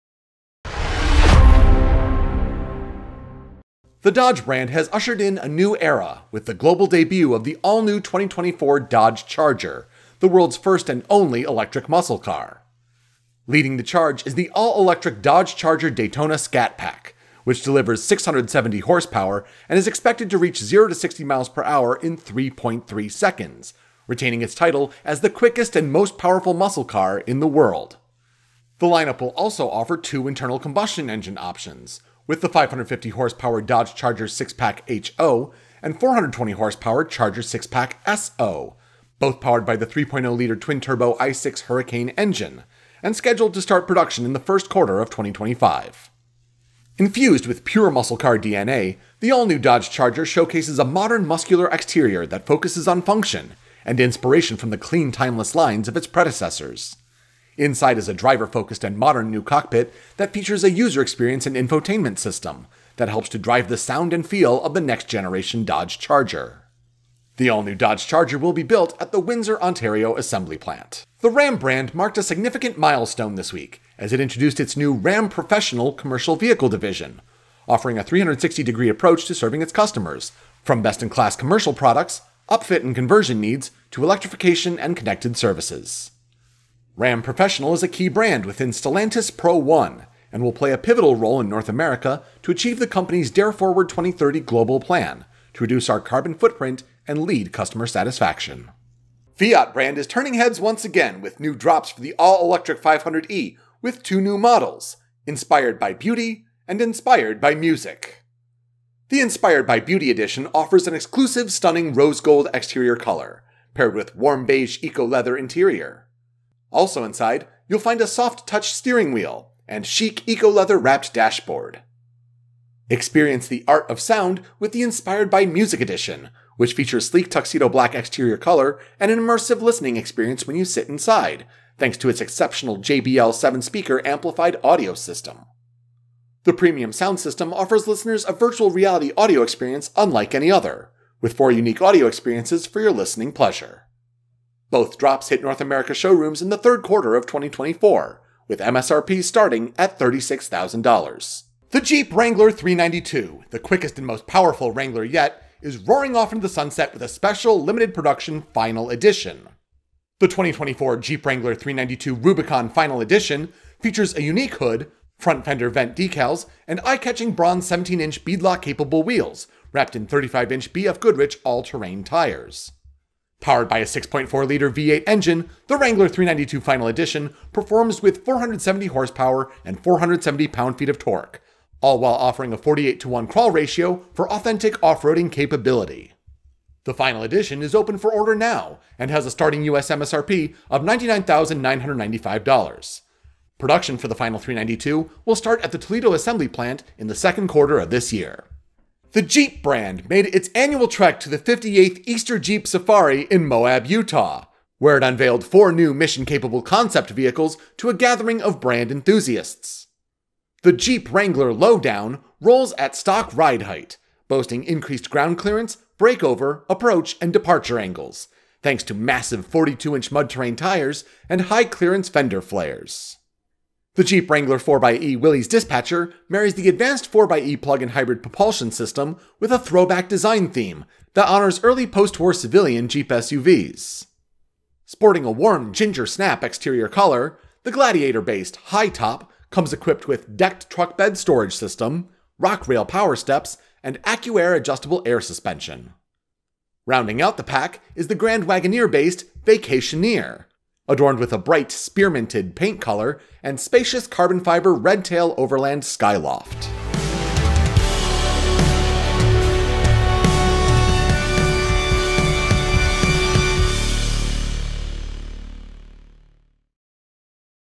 Nuevo Charger | Ram Professional | Nuevos drops del 500e | Wrangler 392 Final Edition |EJS concepts
Repasamos la actividad generada por la compañía Stellantis en el pasado mes. Destacamos la presentación de la nueva generación del Dodge Charger 2024 con variantes electrificadas y motor de gasolina, el lanzamiento de la nueva división de vehículos comerciales Ram Professional, las ediciones Inspired by Beauty e Inspired by Music del Fiat 500e, la edición final del Wrangler 392 y los cuatro conceptos con que Jeep y Mopar acuden a la 58 edición del Easter Jeep Safari.